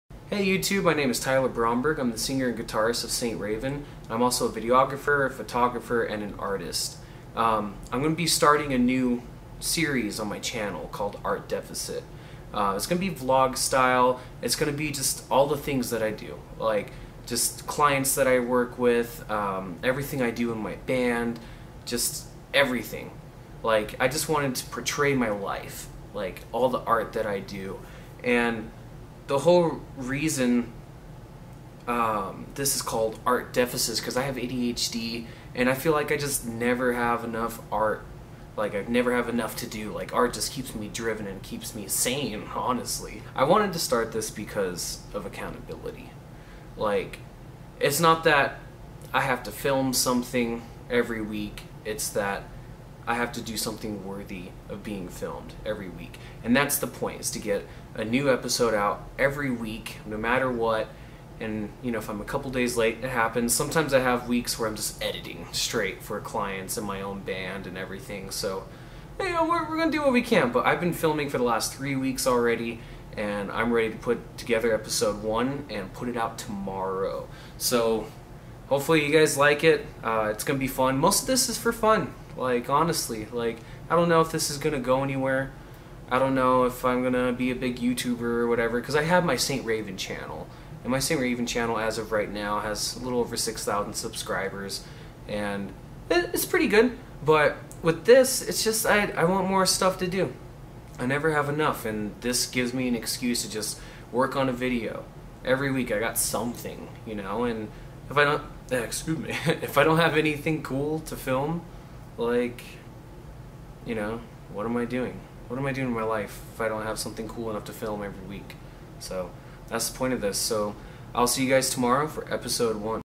Art Deficit Announcement
I'm starting a new vlog called 'Art Deficit'! A big inspiration for doing this has been my friend Conner Christensen making his new channel, 'the975'. He's started a daily challenge for himself and is vlogging it along the way, I highly recommend you check out his channel!